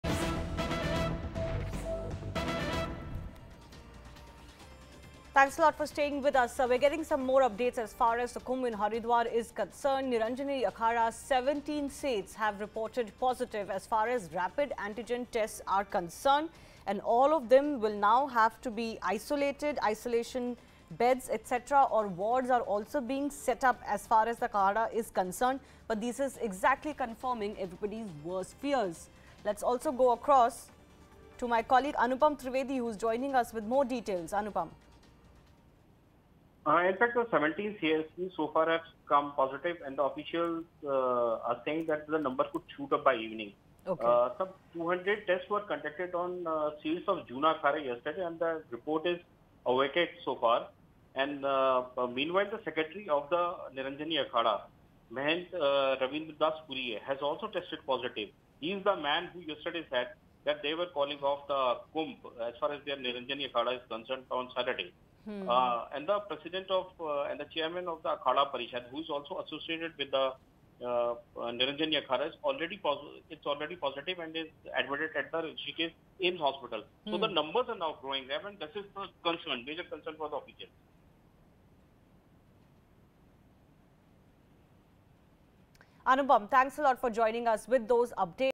Kumbh Covid Crisis | 17 Seers Test Positive | Covid19 Second Wave | CNN News18
Niranjani Akhara's 17 saints have reported positive in the rapid antigen test. All 17 in isolation.